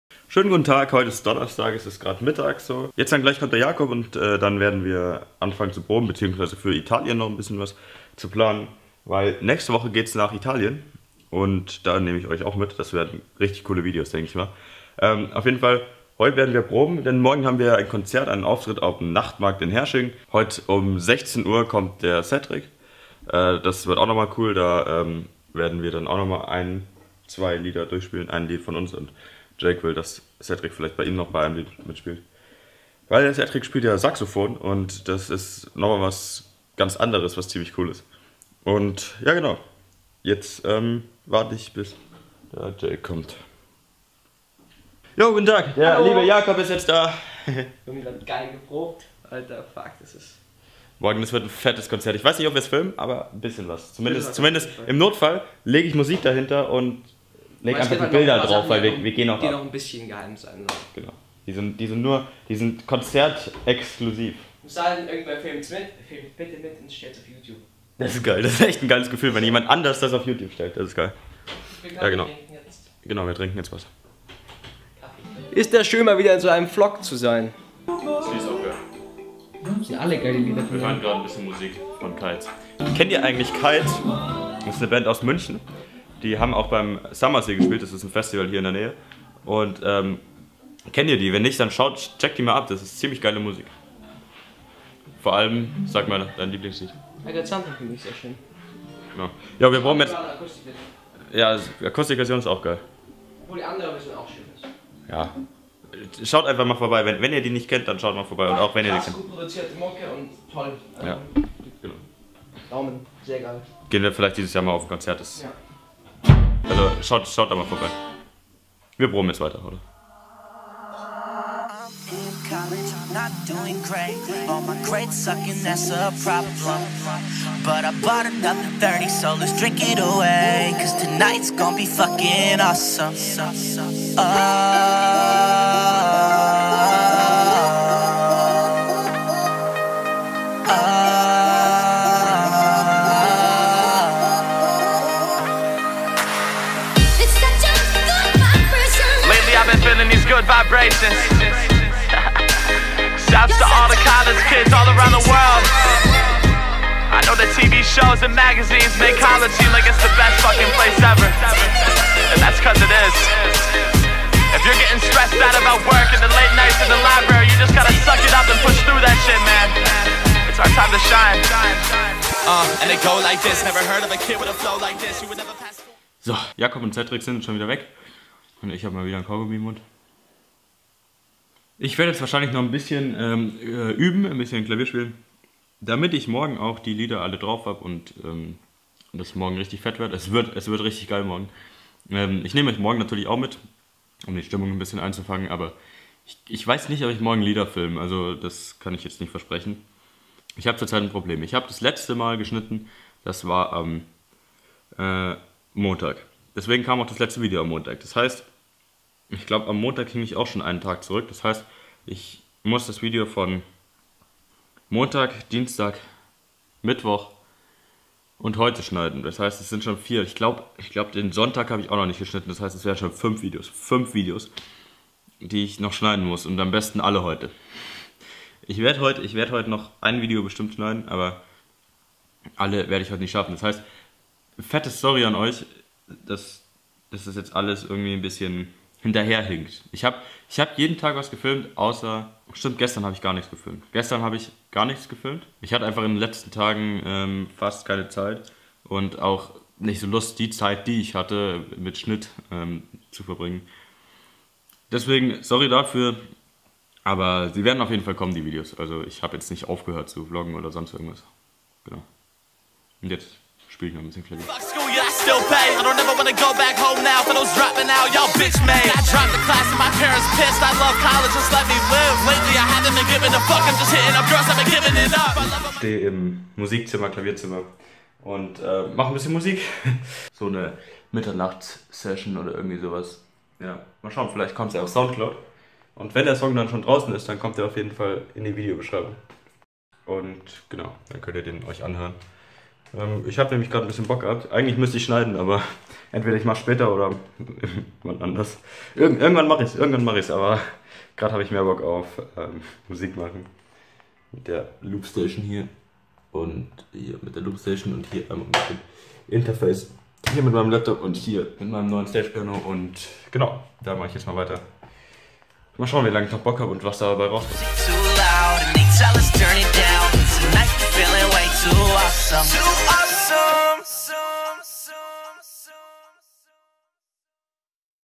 fettes Proben! | Vlog 17
Jake und ich proben mal wieder für ein fettes Konzert unserer Band.
Viel Spaß!